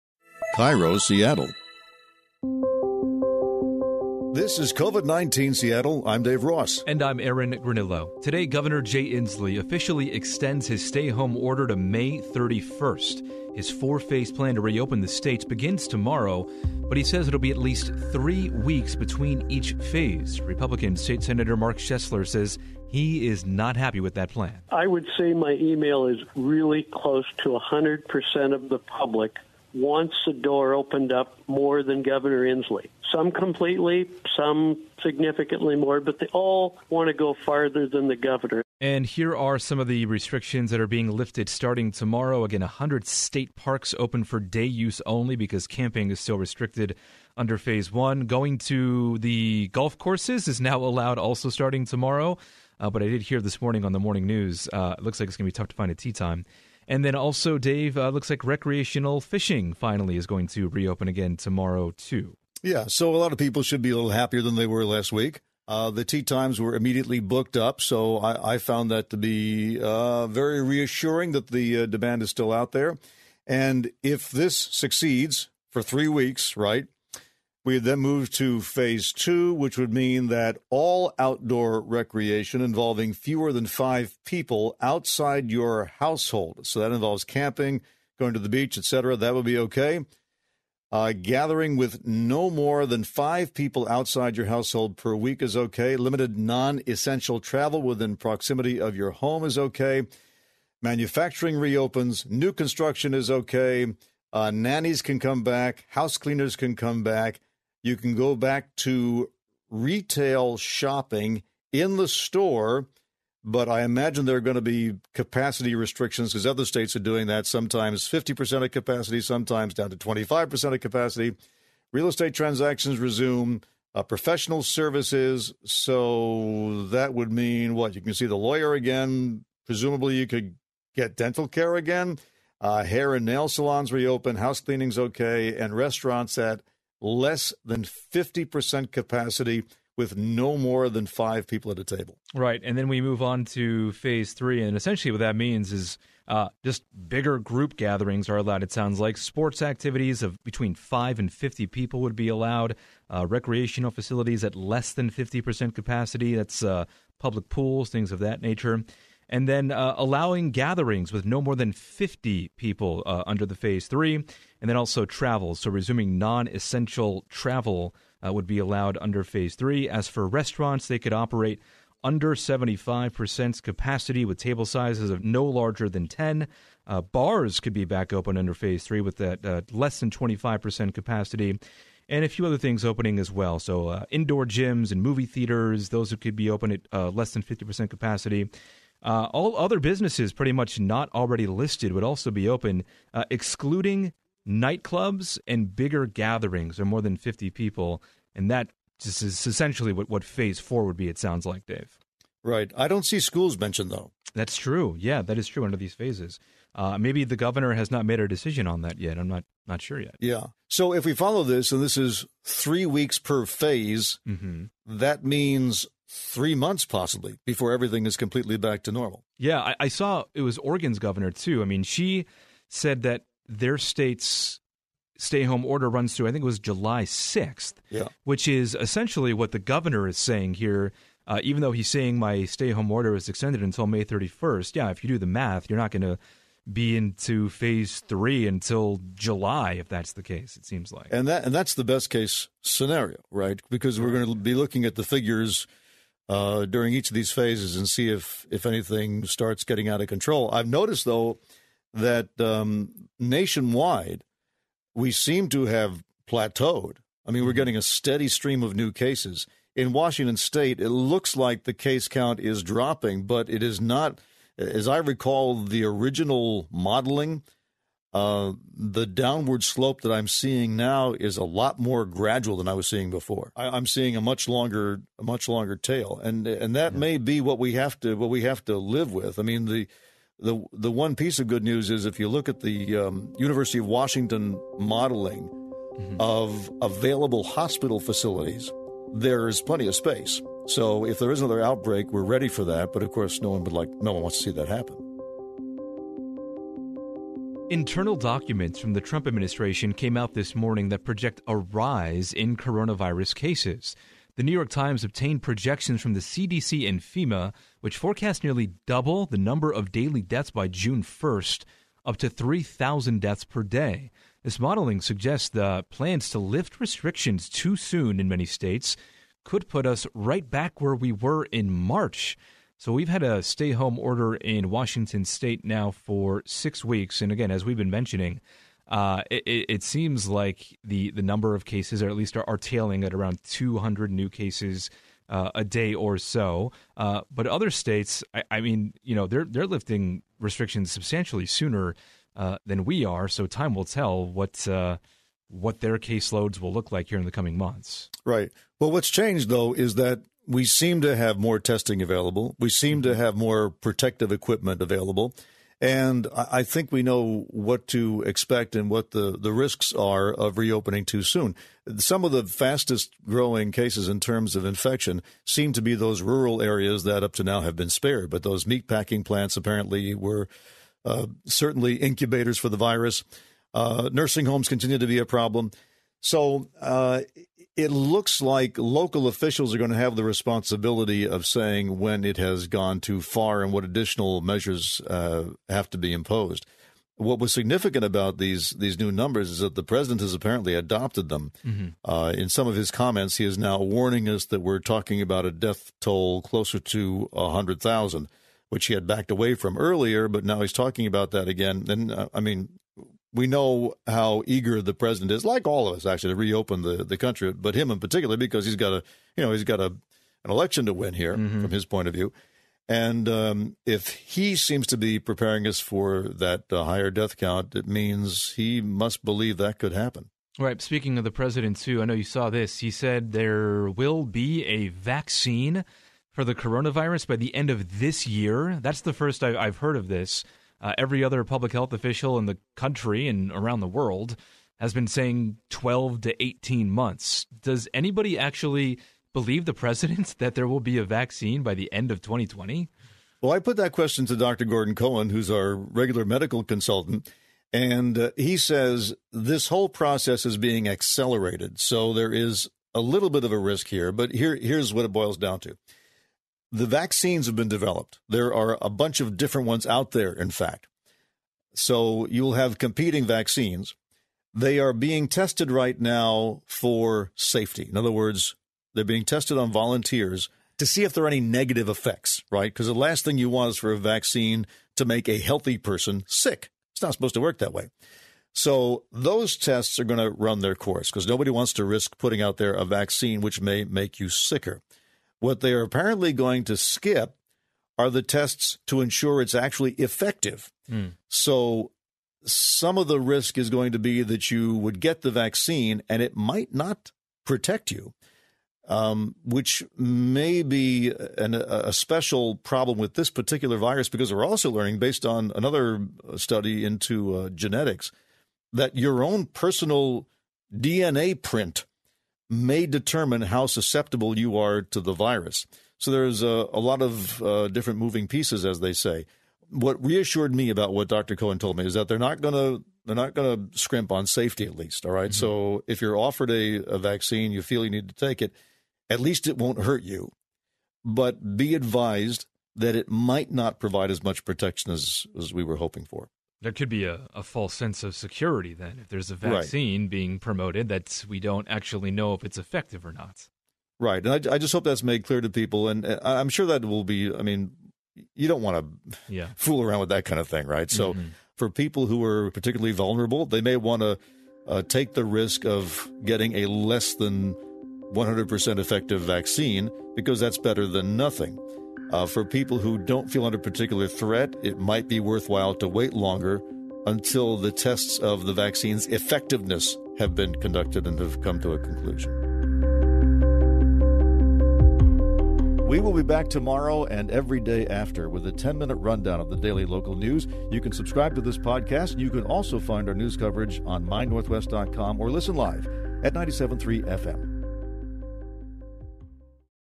The Plan to Reopen Washington, Explained
@KIRO Radio 97.3 FM's Dave Ross and Aaron Granillo explain the plan to reopen Washington State.